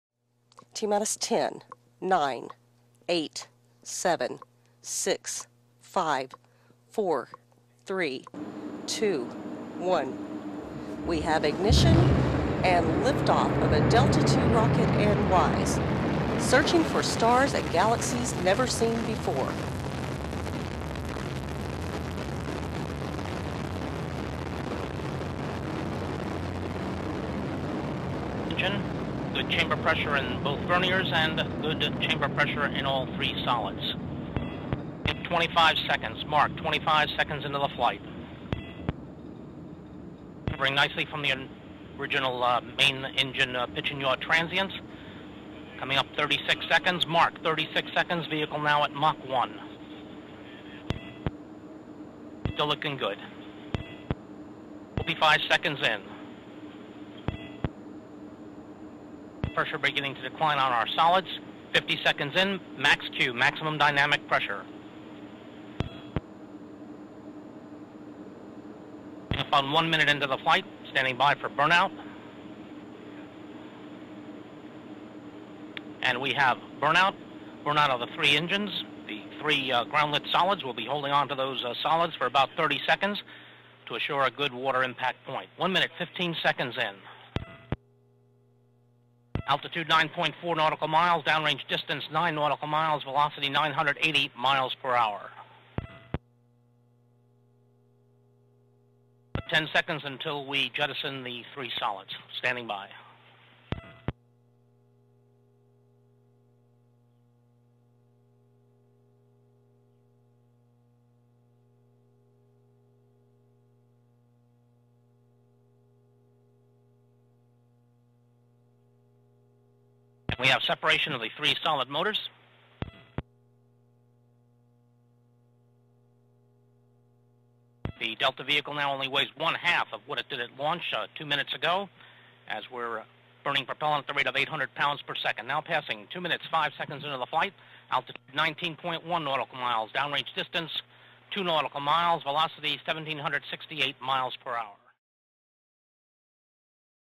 WISE Launch
NASA's Wide-field Infrared Survey Explorer spacecraft lifted off from Vandenberg Air Force Base in California on a mission to map the entire sky at four infrared wavelengths, cataloging hundreds of millions of objects.